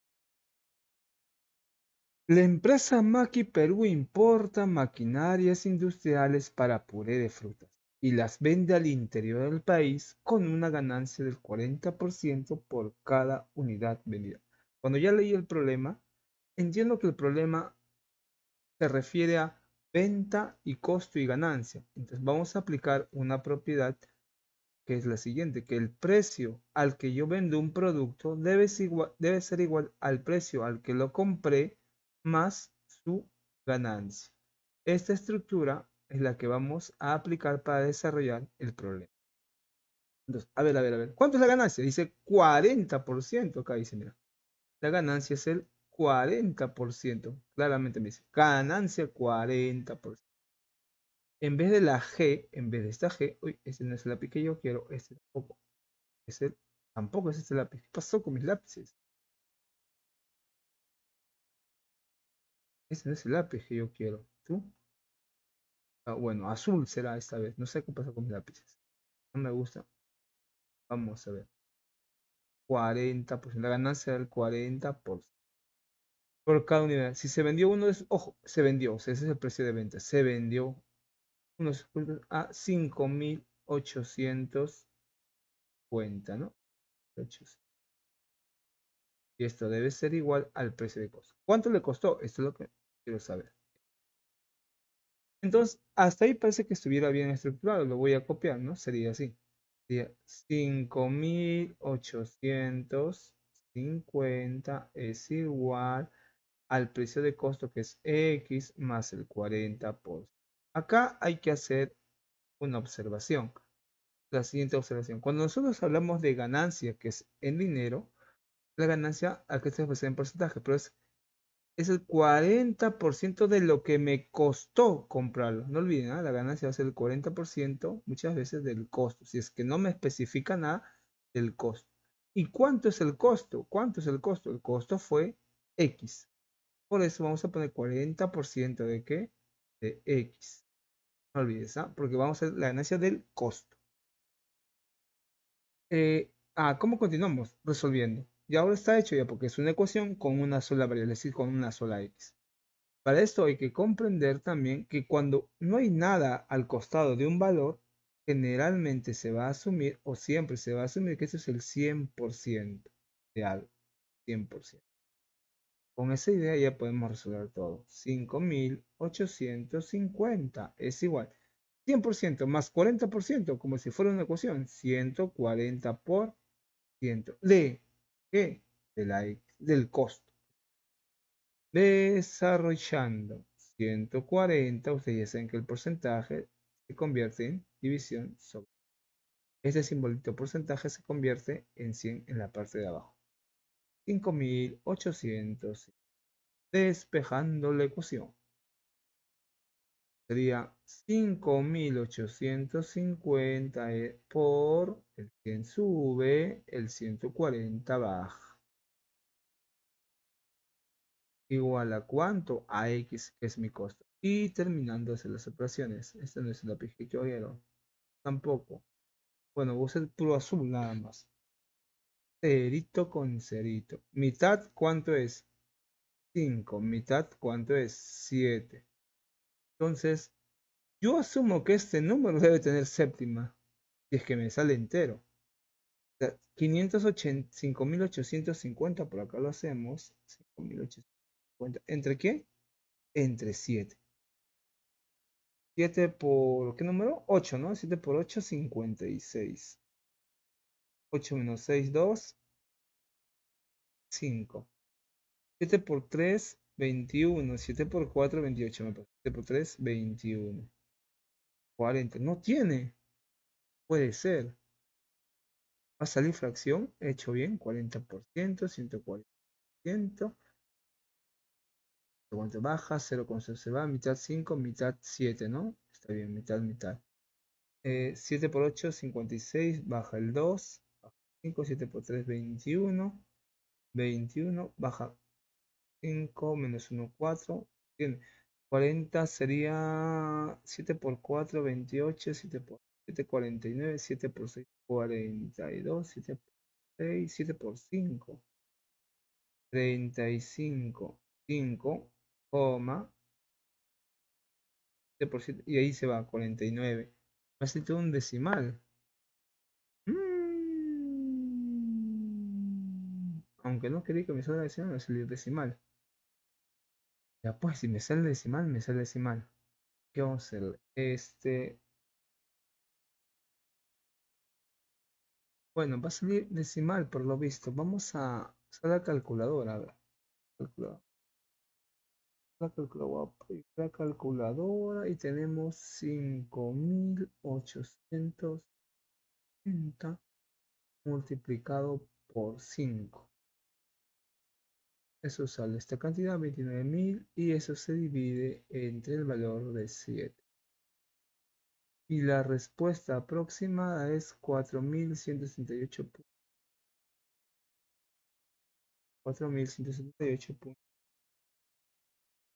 ✅Explicando tareas | 👉 La empresa Maqui Perú importa maquinarias industriales para puré de frutas y
La empresa Maqui Perú importa maquinarias industriales para puré de frutas y las vende al interior del país con una ganancia del 40% por cada unidad vendida. Si se vendió uno de sus productos en 5 850 dólares. ¿Cuánto le costó a la empresa dicha maquinaria?​

Participa enviando alguna duda que tengas en tu tarea para que podamos explicarlo. Apoya a otros usuarios en sus dudas.